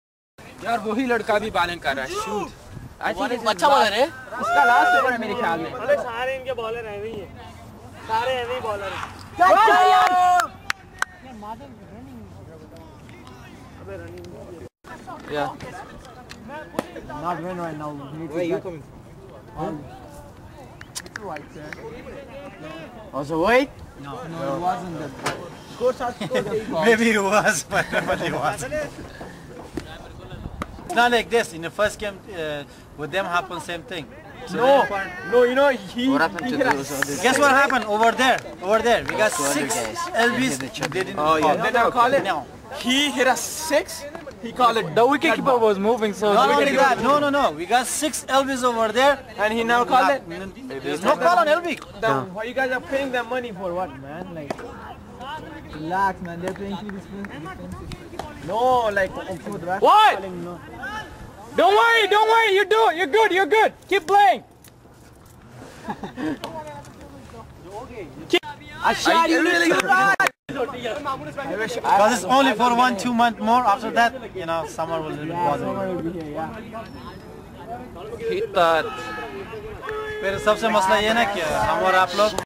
EY vs. Central (2nd match) - Last Few Moments
East York vs. Central Commerce

These are the last few moments of EY batsmen trying to finish the game and doing their best to complete the target. Fortunately, we won the game.